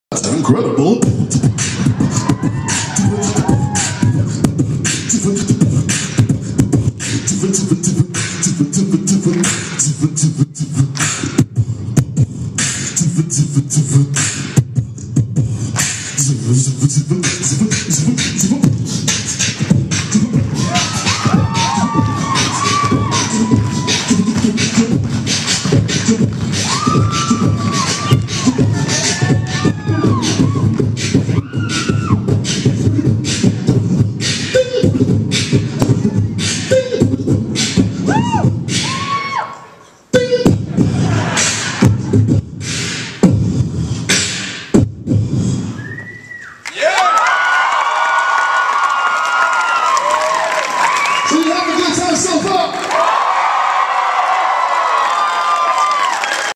Beatbox Solo Scratch Track St.Paul 2011 NACA
Scratch Track's DJ Lee performs a beatbox solo for the students and advisors at the Northern Plaines NACA in St.Paul MN.